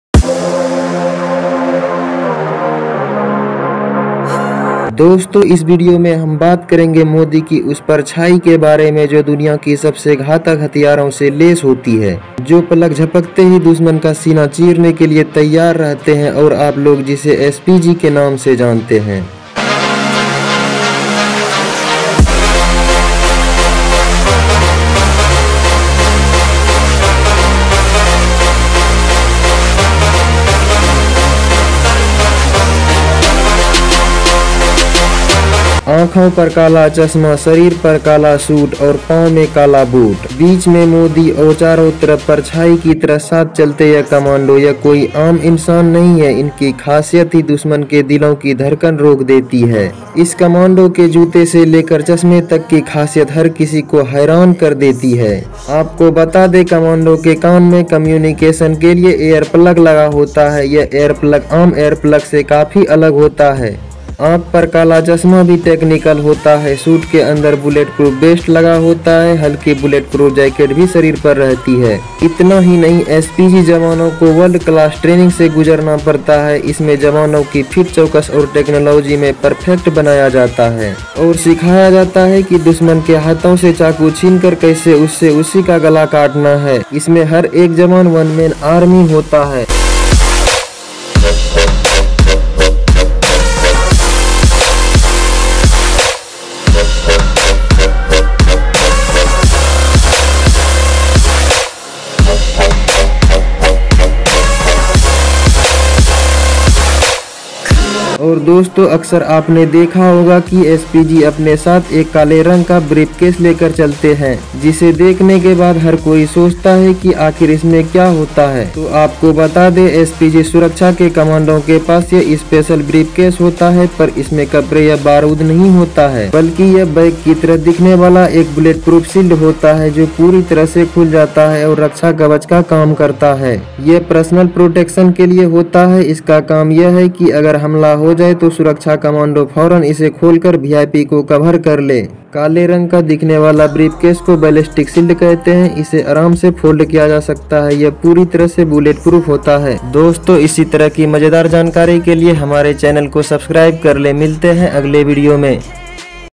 दुनिया के सबसे खतरनाक हथियार से लैस PM की सुरक्षा में तैनात 3000 SPG कमांडो की पूरी कहानी। 🤯
दोस्तों इस वीडियो में बात करेंगे दुनिया की सबसे खतरनाक हथियार से लैस पीएम की सुरक्षा में तैनात एसपीजी कमांडो की ।

तो आप सबसे निवेदन है की वीडियो में लास्ट तक बने रहे।

एसपीजी कमांडो की पूरी कहानी 😀